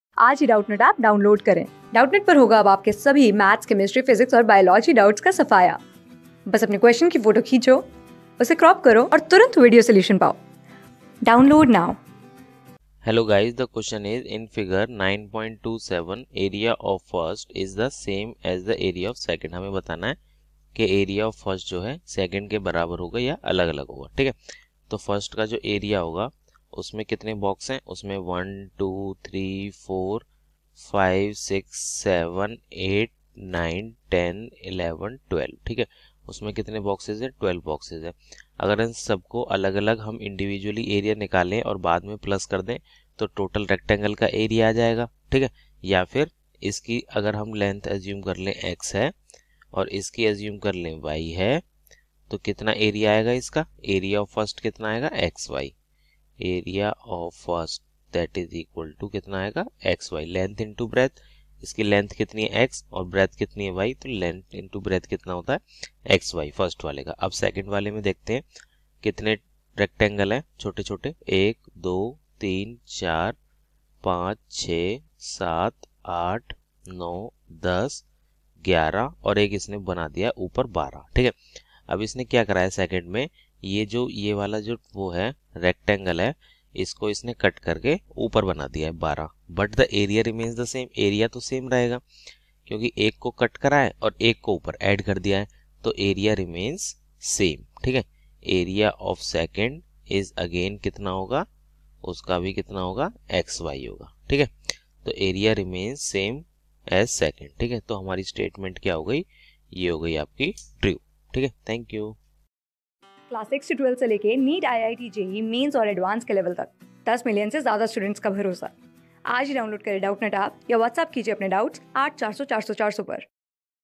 In Fig. 9.27 area of (i) is the same as the area of (ii). | CLASS 7 | PERIMETER AND AREA | MATH...
In Fig. 9.27 area of (i) is the same as the area of (ii).
Class: 7
Subject: MATHS
Chapter: PERIMETER AND AREA
Board:CBSE

👉 Have Any Query? Ask Us.